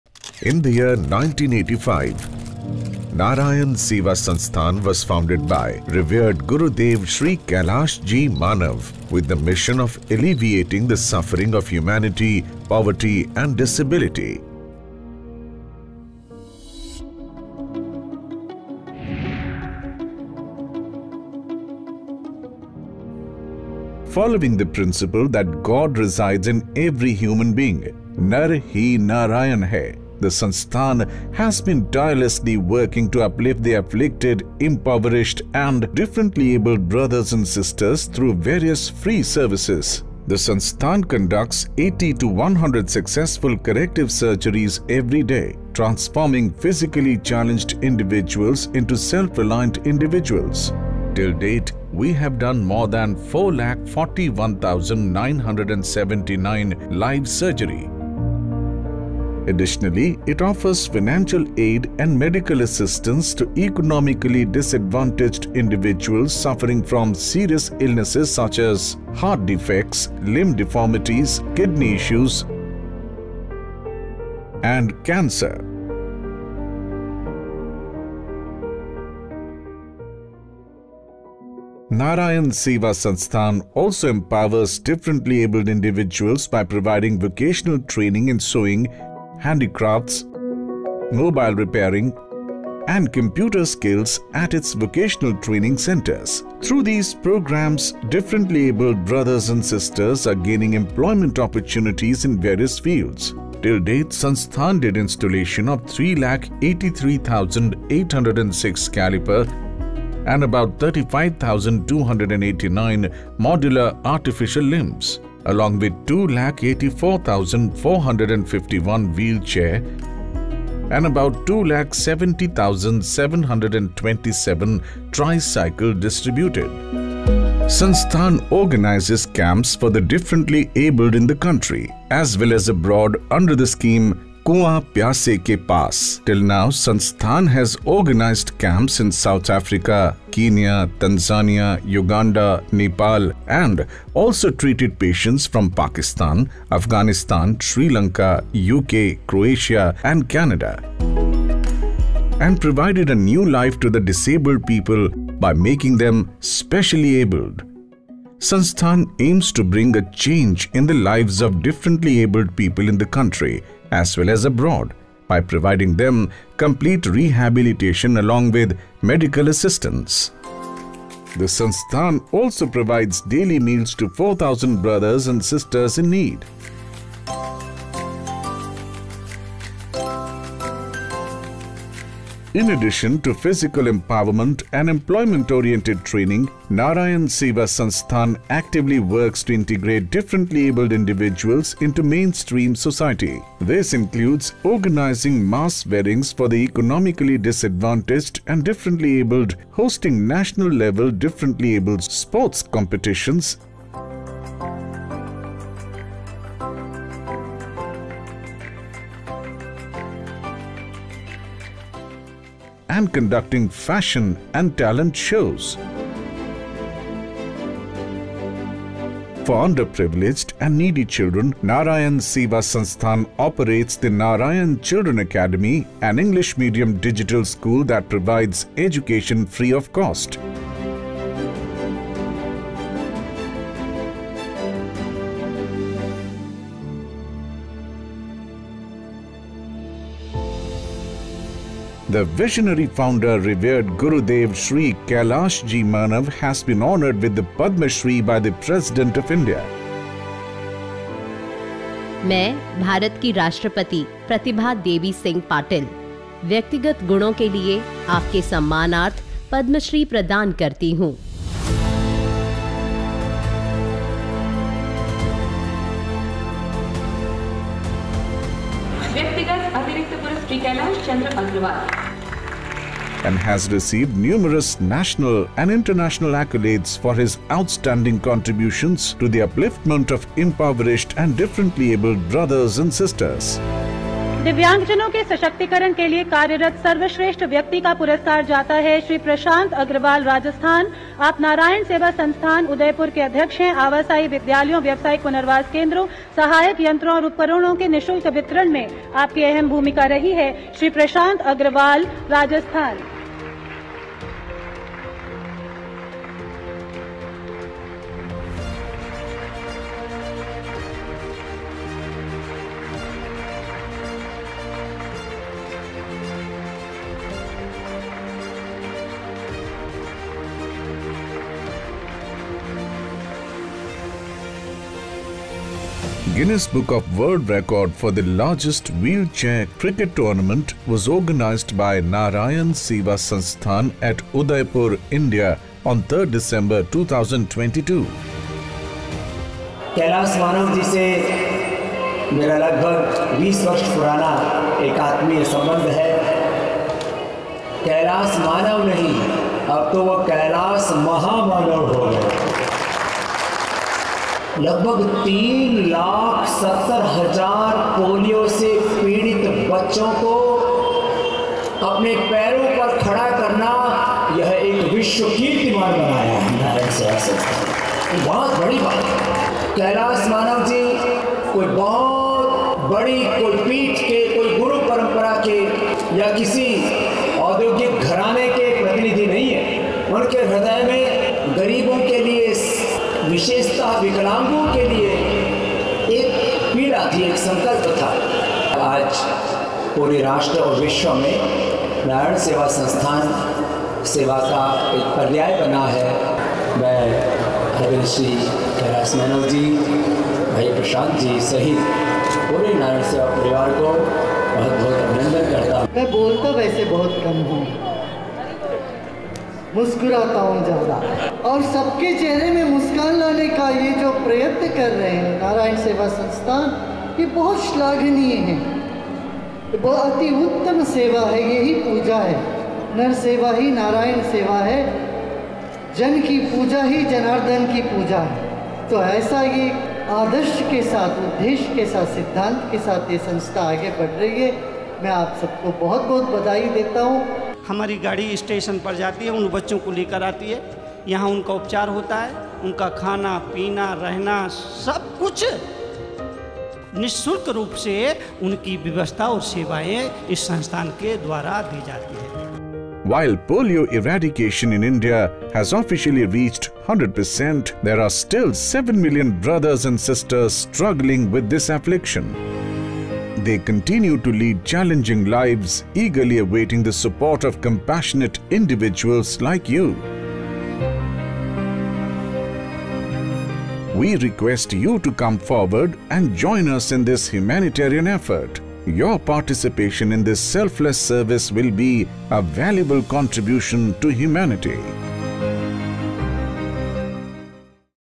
Differently-abled people are getting new heights | Narayan Seva Sansthan | Documentary | Best NGO
Established in 1985, Narayan Seva Sansthan has been a beacon of hope for persons with disabilities for the past 39 years. This documentary showcases the sansthan's remarkable journey, having performed over 4,42,000 free corrective surgeries and providing artificial limbs and assistive devices to those in need.

Beyond medical aid, Narayan Seva Sansthan extends its support to countless helpless, destitute, and differently-abled individuals through various initiatives. Dedicated for the Orphanage for orphans, Skill Development Centre, Physiotherapy Centre, Narayan Children Academy, and the Mass Marriage Program, all to uplifting and transforming lives. Join us in celebrating the unwavering commitment to serving suffering humanity.

⏩ Check out the शिव महापुराण कथा : पूज्य पं. विजय शंकर जी महाराज द्वारा

Follow our various Social Media Channels :-